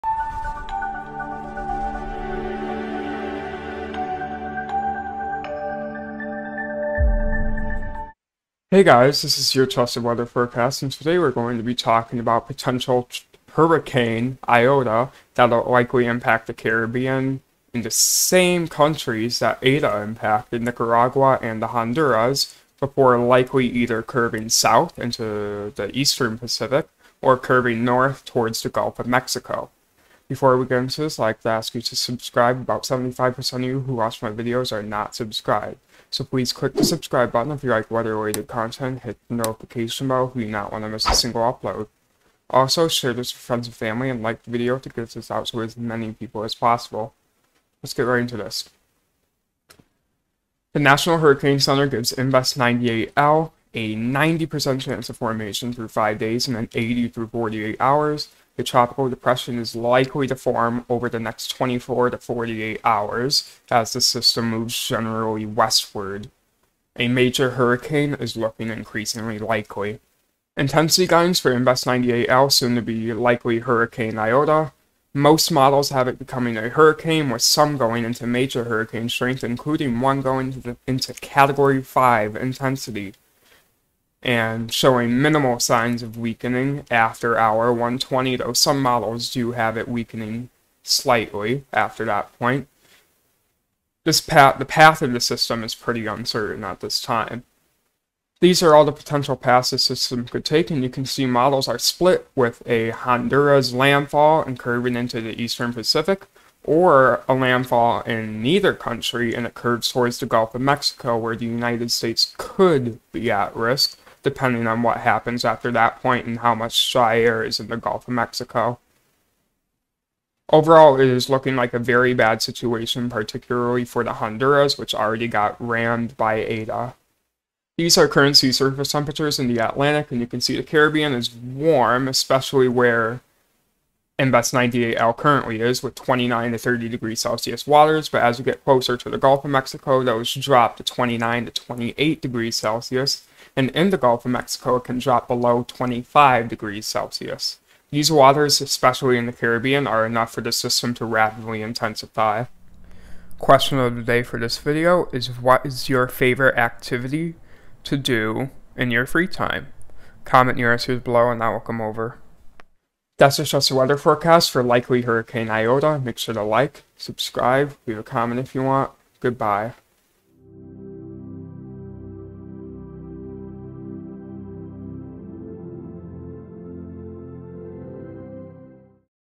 Likely Hurricane Iota Forecast
Invest 98L will likely become hurricane Iota as it aims towards Nicaragua and the Honduras, where it will likely bring more devastating landfall and extreme storm surges to the countries. There is a small chance this system curves towards the Gulf of Mexico and puts the U.S at risk, otherwise most models agree on it curving into Central America and leaving in the pacific,

Question of the day: What is your favorite activity to do in freetime?